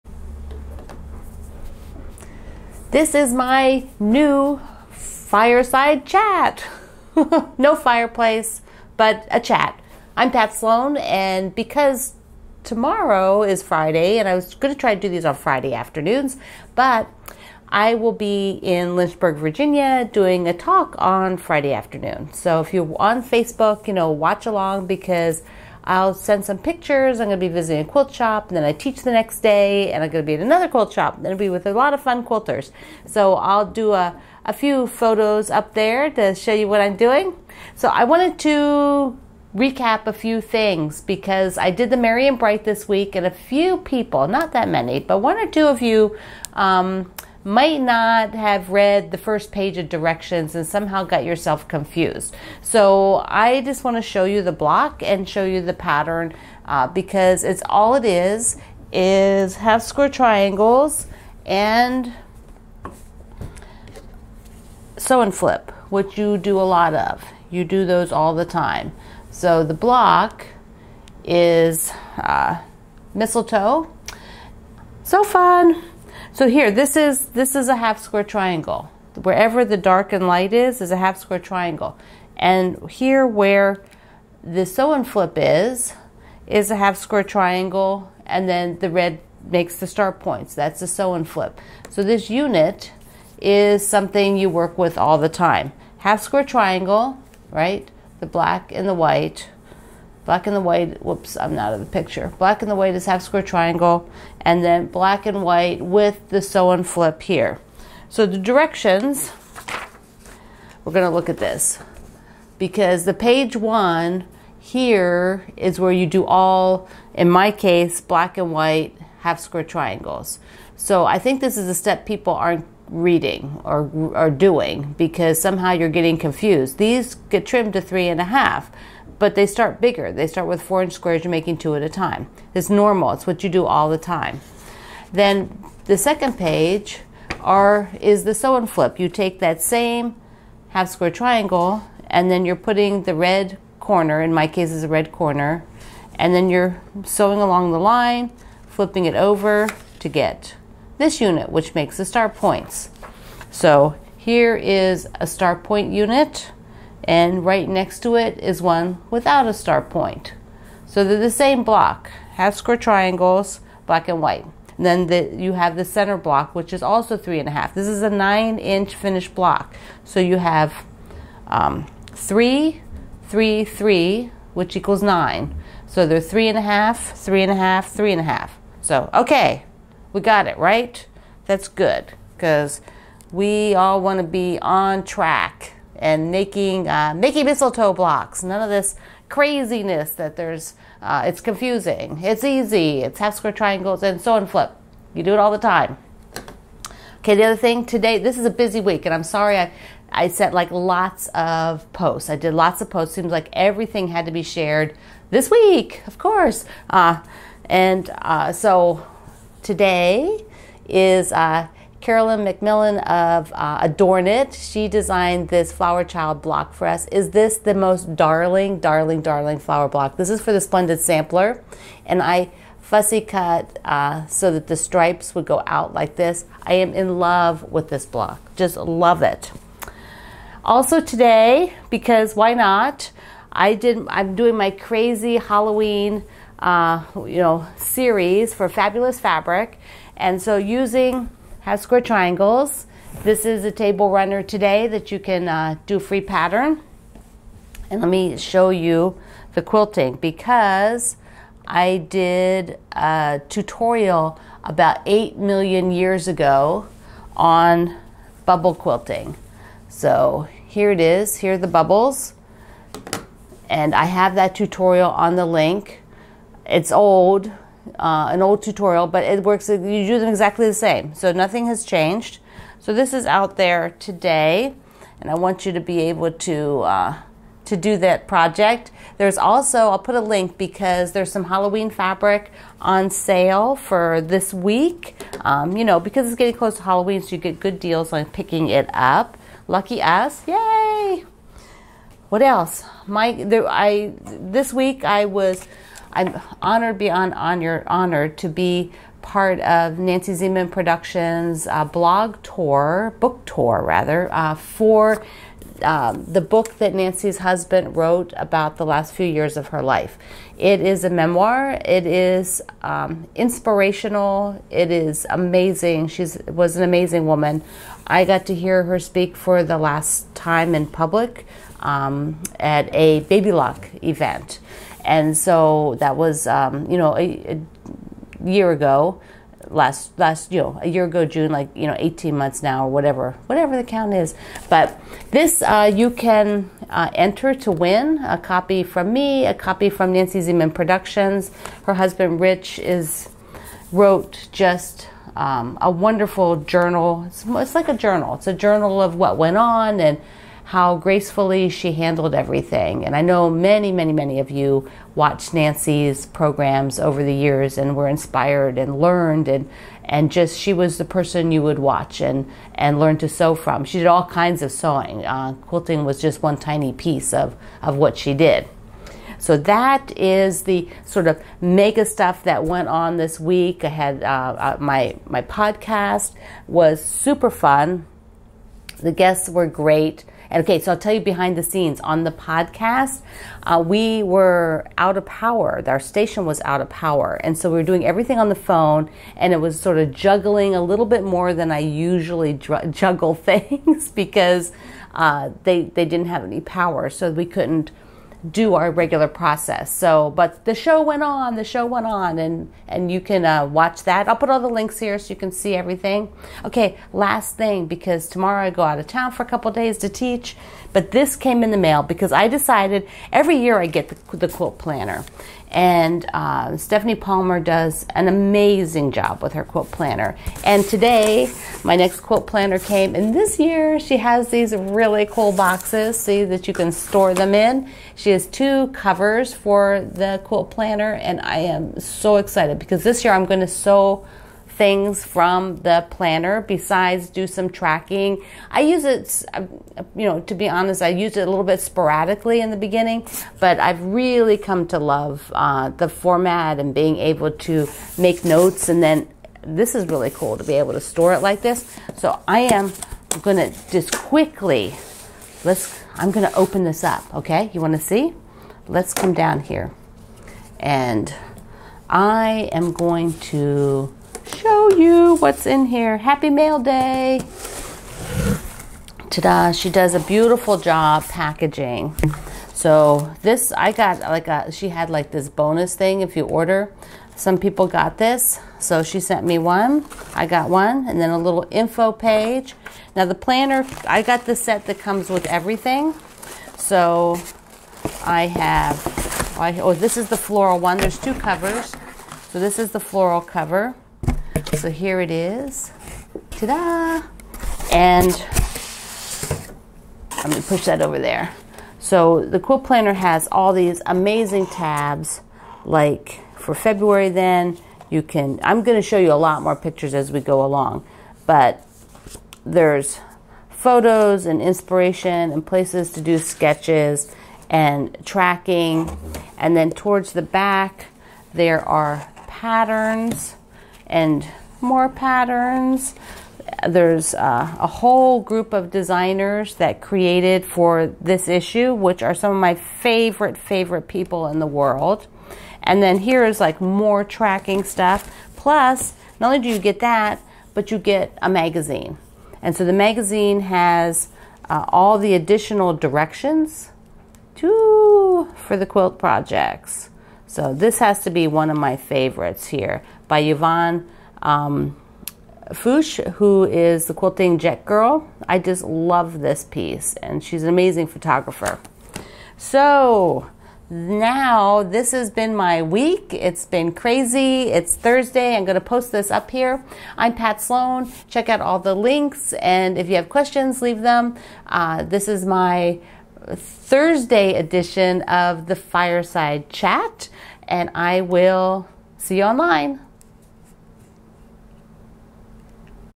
Thursday edition of Pat's Friday Fireside Chat
((Thursday edition of  Pat's Friday Fireside Chat)) This chat is being done Oct 18, thanks for joining me! I'm talking about the projects I'm working on
**Enter to win a copy of Nancy Zieman, The rest of the story